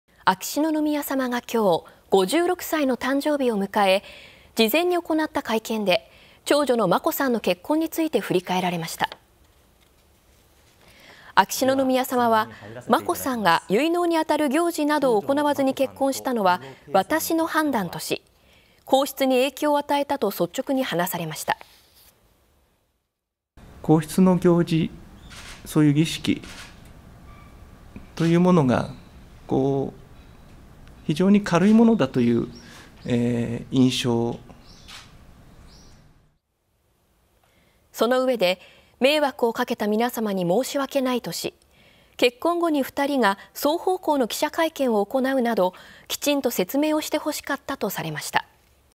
秋篠宮さま56歳で会見 眞子さん結婚で「皇室儀式が非常に軽く」（2021年11月30日）
これはショートニュースです。ノーカット版会見と解説「娘との楽しかった思い出」↓

秋篠宮さまがきょう56歳の誕生日を迎え、事前に行った会見で長女の眞子さんの結婚について振り返られました。
秋篠宮さまは眞子さんが、結納に当たる行事などを行わずに結婚したのは「私の判断」とし皇室に影響を与えたと率直に話されました。

秋篠宮さま「皇室の行事、儀式が非常に軽いものだという印象を…（与えてしまった）」。

その上で、迷惑をかけた皆様に申し分けないとし、結婚後に２人が双方向の記者会見を行うなどきちんと説明をしてほしかったとされました。